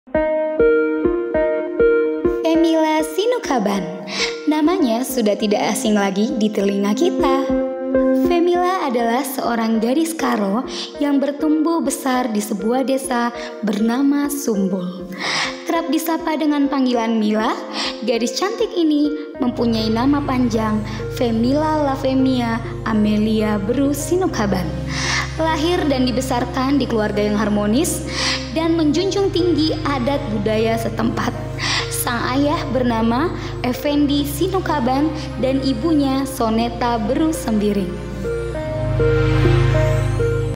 "The Journey of Femila Sinukaban" - Chapter 1
"The Journey of Femila Sinukaban"
Chapter 1 - "Femila"
Siapa Femila? Cerita tentang Keluarga dan Latar belakang Femila yang tidak diketahui banyak orang